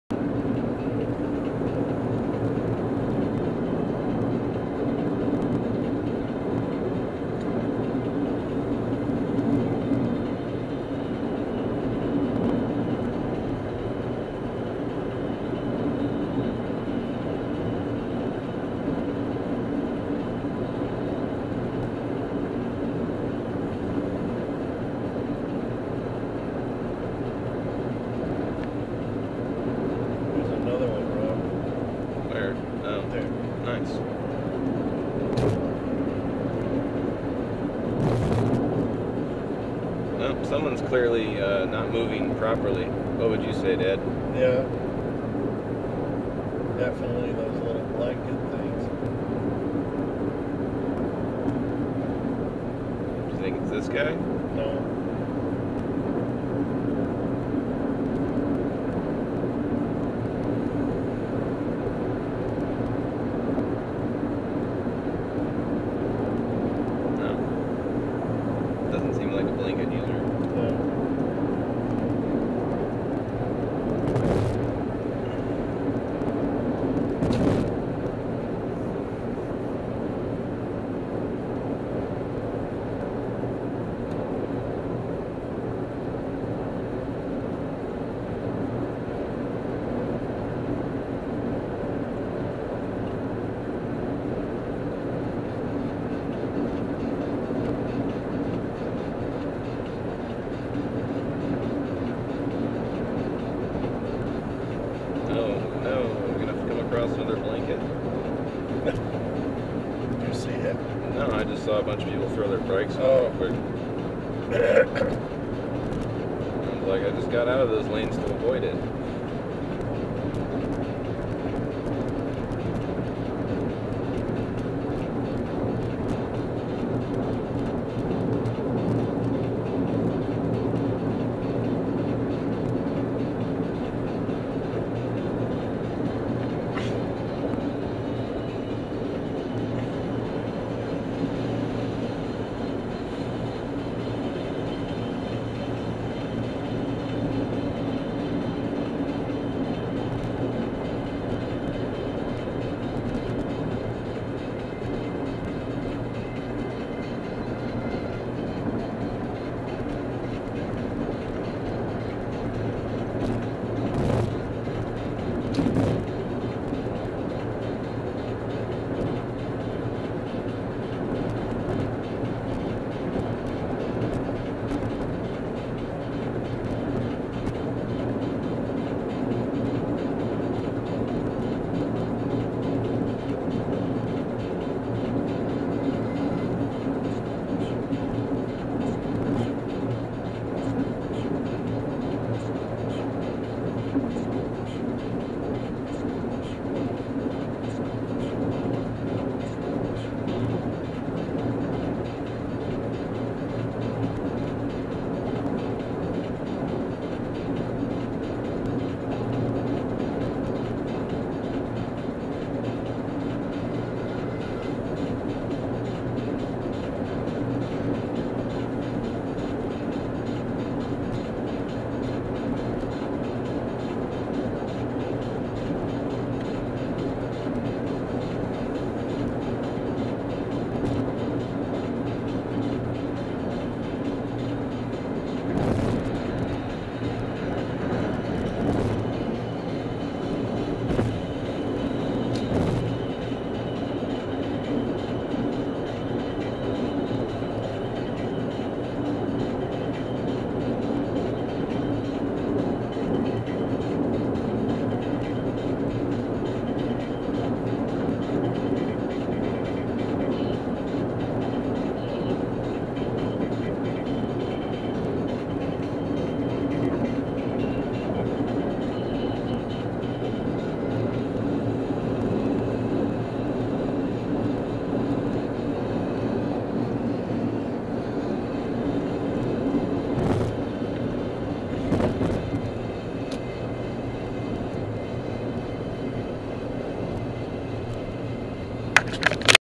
2012.05.05 Daily Driving: Food, Airport /w Dad [4/11]
Got some food, and then took my dad to the airport today. Check out the nature lover driving a Caddy in the Fuddruckers parking lot. Driving around now with MORE commentary! Sometimes even special Guests. Just an example video from my Contour HD camera to show off the video quality to friends. You might want to turn down your speakers, the microphone is lacking.